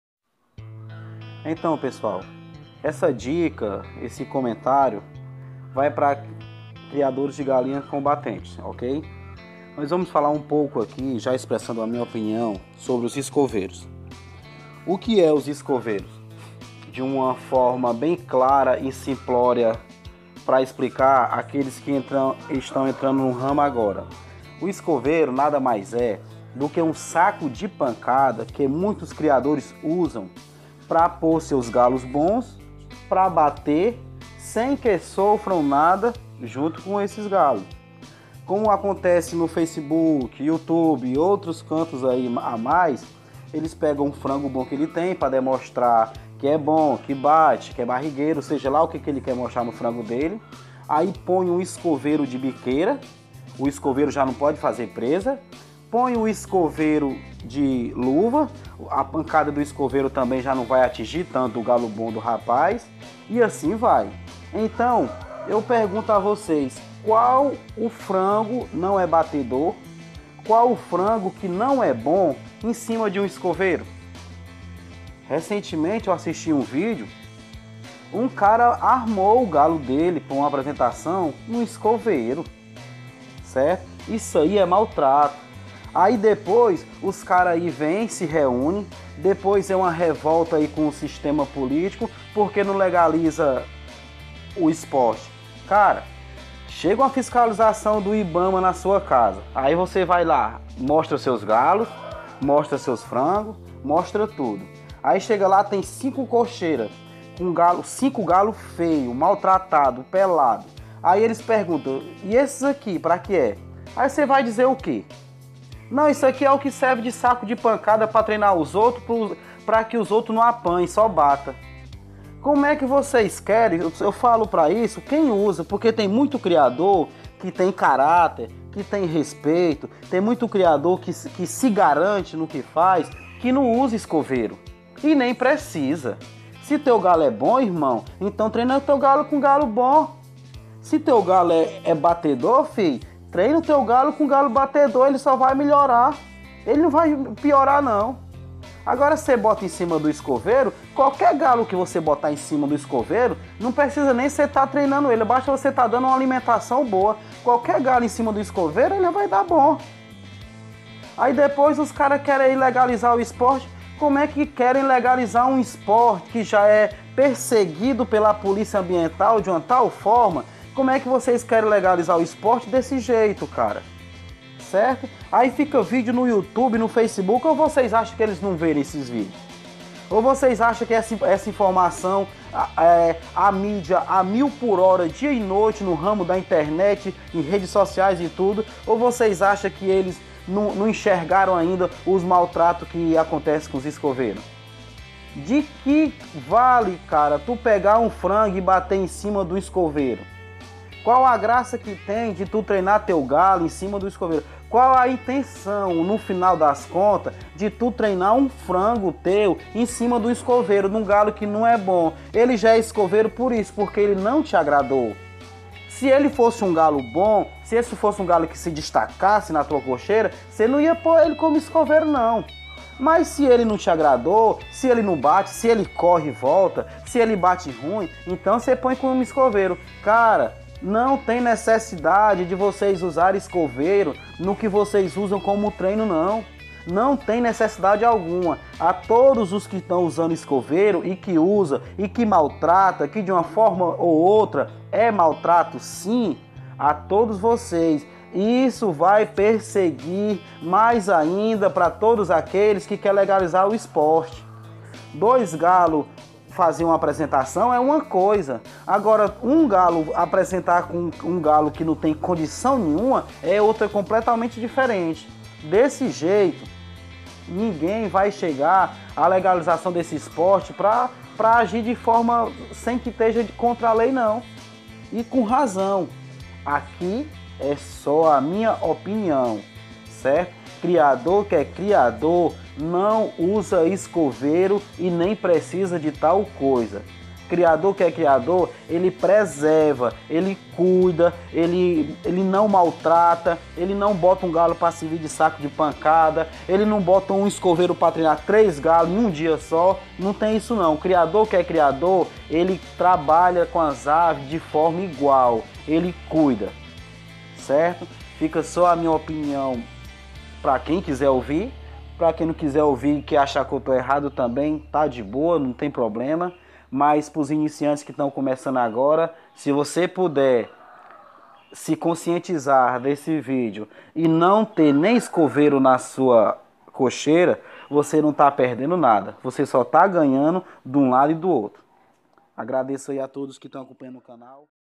✅ Opinião sobre escoveiros - Criatório torres
✅  neste vídeo falo um pouco sobre minha Opinião sobre escoveiros - Criatório torres

  ✅canal para criadores iniciantes da raça índio combatentes, com dicas e manejos.